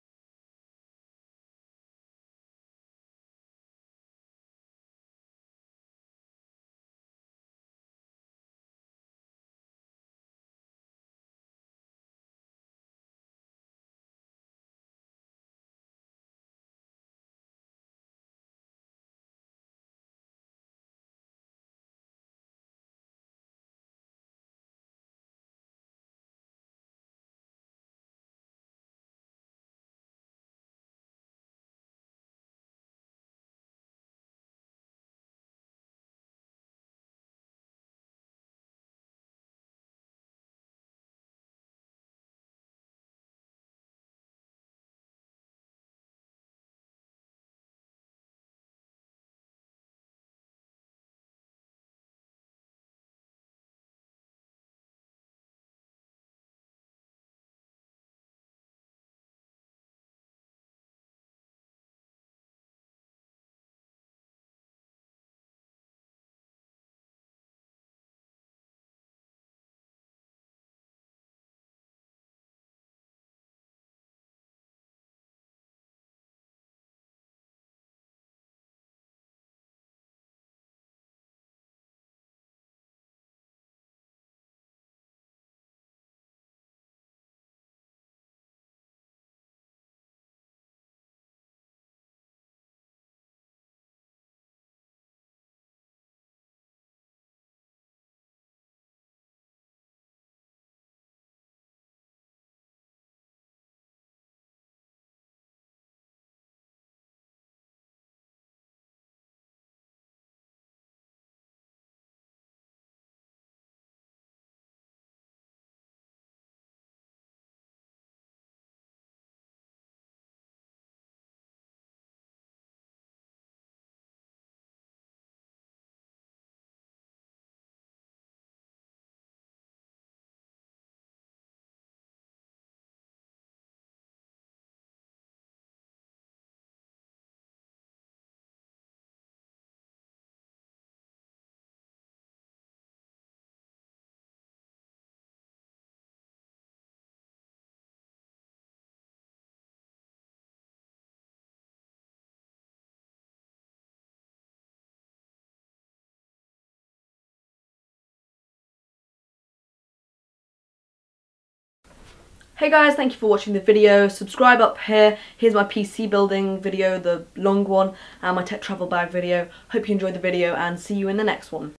pc build under $1000 (timelapse)
this is the timelapse version of my build for those people who want a shorter video, hope you enjoy it! sorry i've tried to get sound on the timelapse but it interferes with the end audio.

UK parts:

cpu cooler

gpu( not included in overall cost since ihavent got it yet)

US parts:

cpu cooler

psu( cheaper one but equally as good, just because the otherone costs too much in the us)

ssd( alternative)